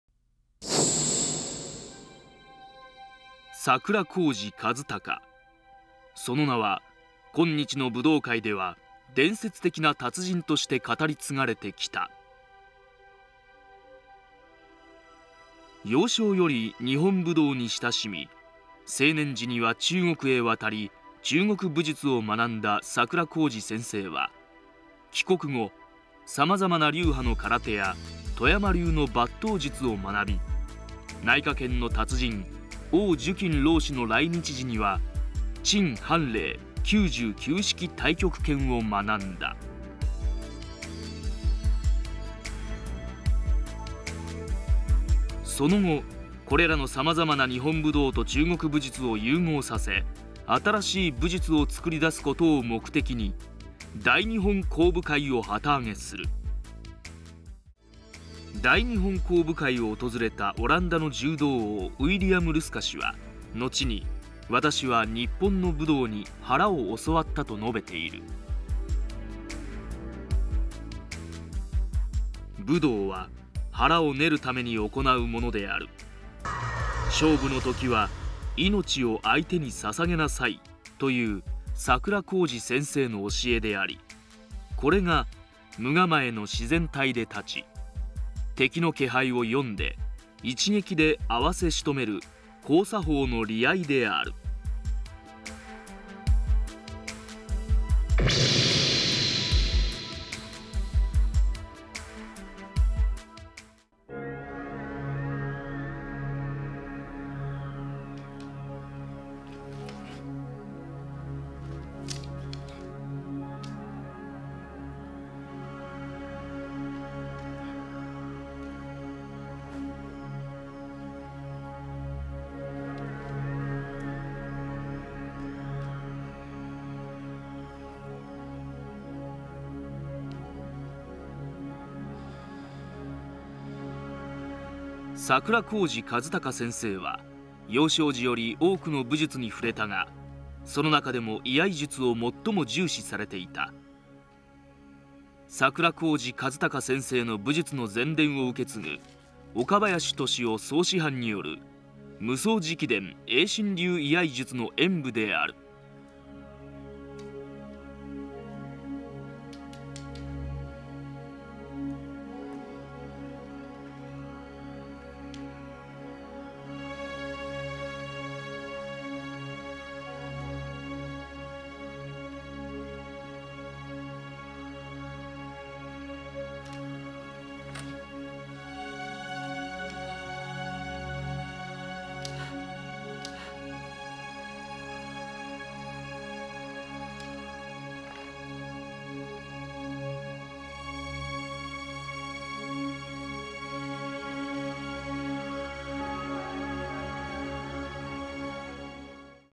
岡林俊雄 嫡流真伝中国正派拳法
DVD「岡林俊雄　嫡流真伝中国正派拳法」のサンプルムービー

（内容説明）
昭和が生んだ伝説的武人、故・桜公路一顱が、中国で学んだ各流派の拳法と王樹金老師から学んだ内家拳の攻防原理に、日本の居合術や古流柔術、伝統派空手の理合いを加味して独自に編成、創り上げた大日本講武會の技術体系。それは、伝統の型にとらわれず、交叉法を中心とした真に実戦的な武術技法のみを追求した画期的なものであった。これは、桜公路一顱が遺した素晴らしい武術遺産を、全伝を受け継ぐ岡林俊雄師範が公開したものである。